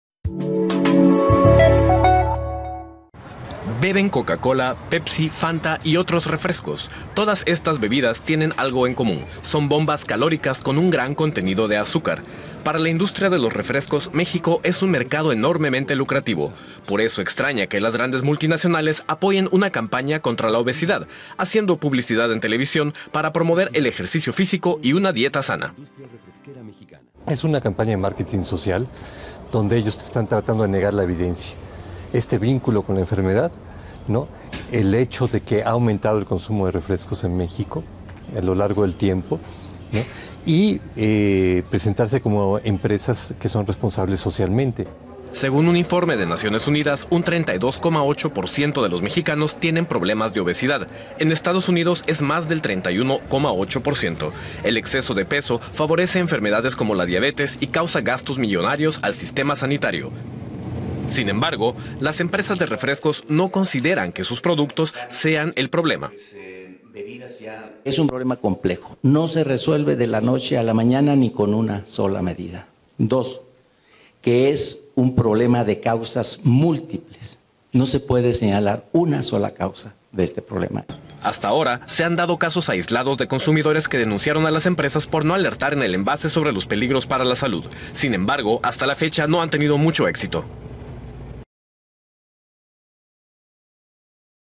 Coca-Cola lanza campaña contra la obesidad en México | Journal
Según un informe de la ONU, un 32,8 por ciento de los mexicanos tienen problemas de obesidad, superando a EE.UU. que muestra cifras de 31,8 por ciento. En México una campaña quiere contribuir a un cambio de costumbres entre la población. Pero los que lanzan la campaña no son sinónimo de una alimentación sana.